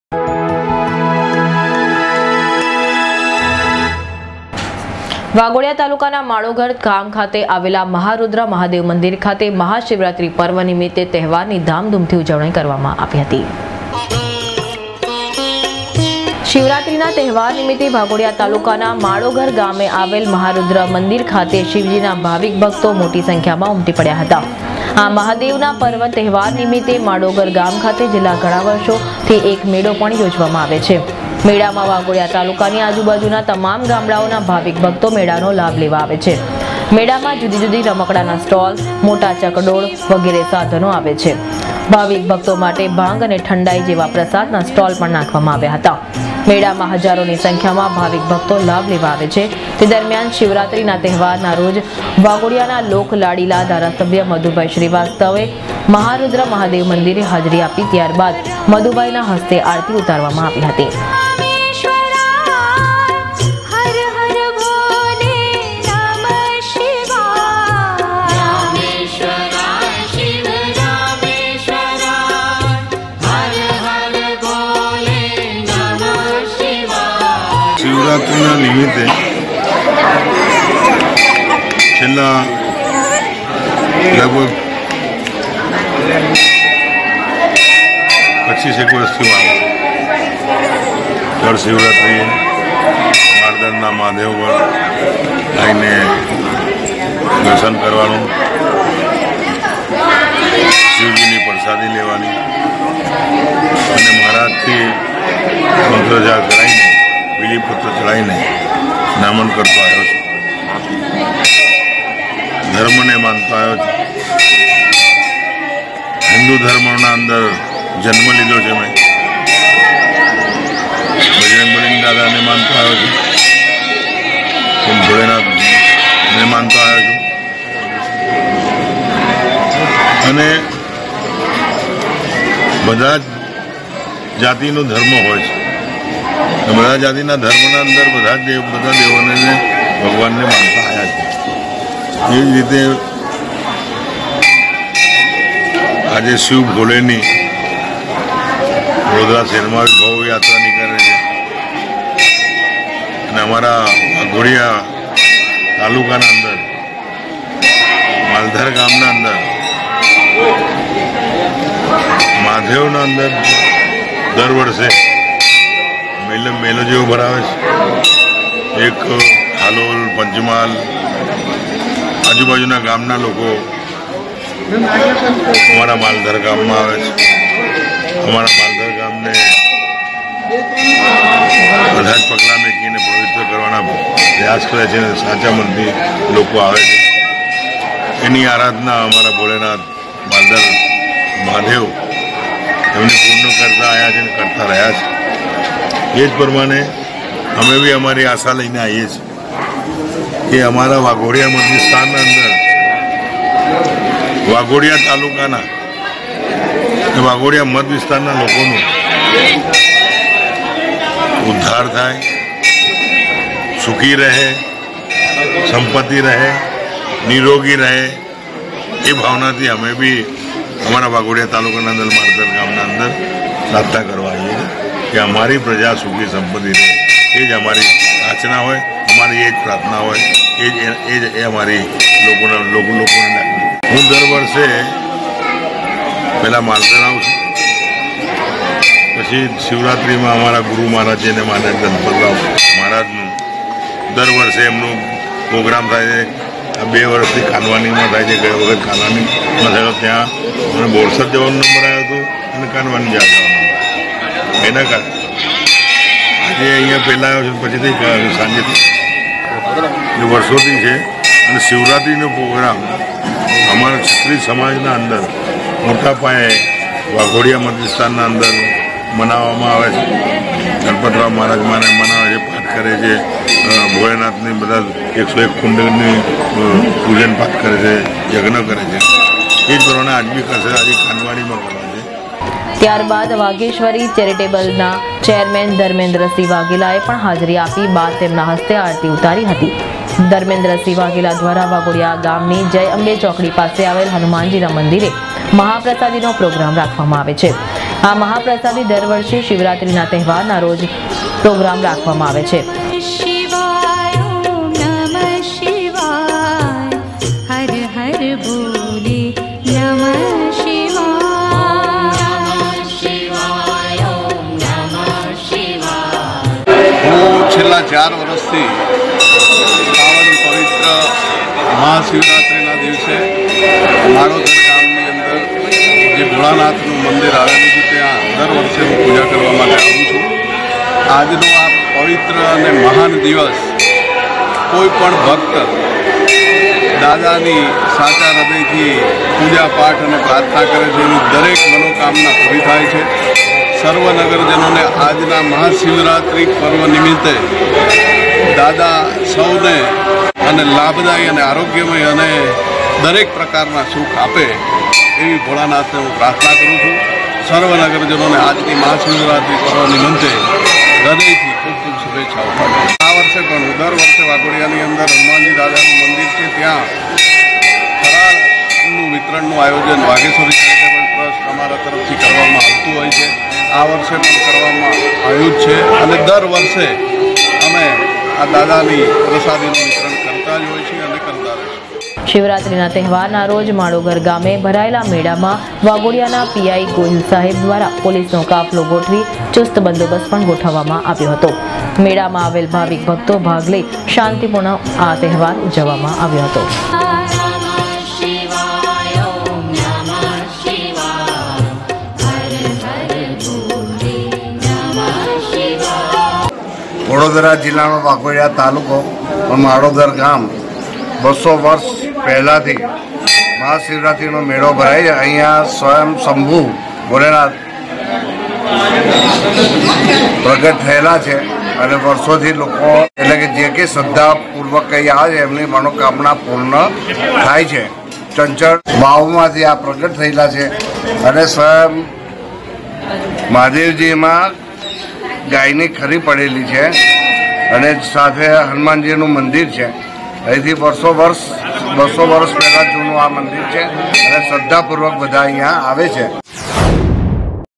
Maharudra Mahadev Temple Waghodia
DNTV NEWS CHANNEL :- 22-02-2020 ( વાઘોડિયા )

વાઘોડિયા તાલુકા ના માડોઘર ગામ ખાતે આવેલ મહારૂદ્ર મહાદેવ ના મંદિરે શિવરાત્રી નો તહેવાર ઘામ ઘુમ થી ઉજવવામાં આવે છે